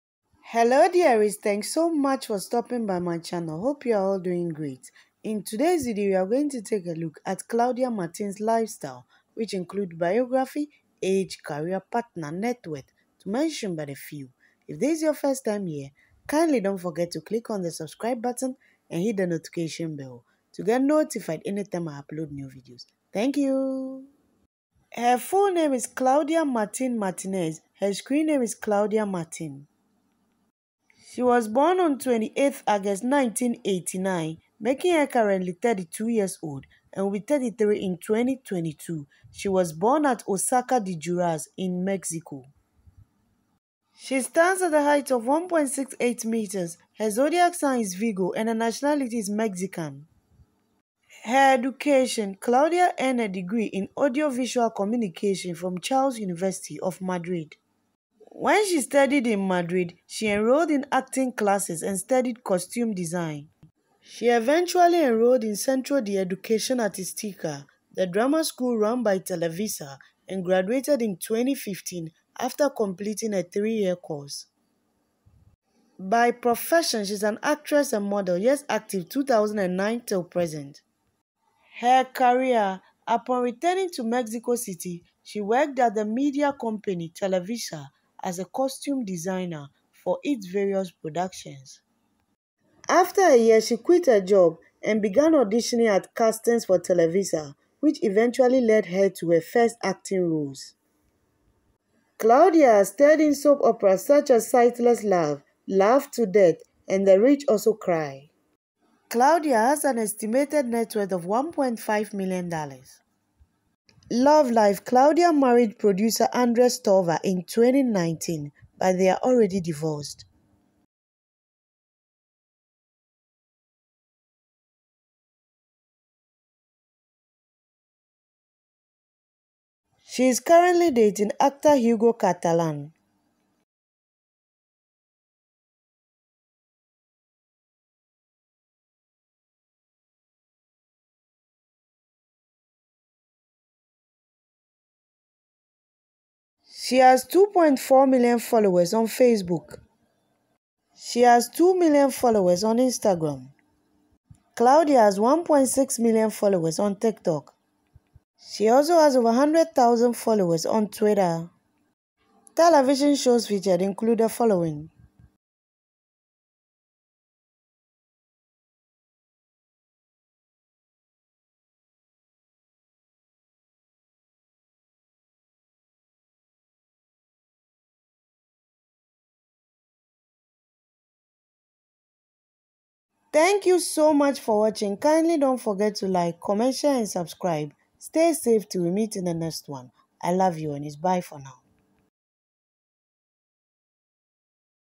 Claudia Martín (Eva) Lifestyle 2022 || Boyfriend, Biography, Career, Net worth
This video gives the details of the lifestyle(Biography, Real Name, Age, Partner, Career and Networth) of Popular Mexican Actress Claudia Martín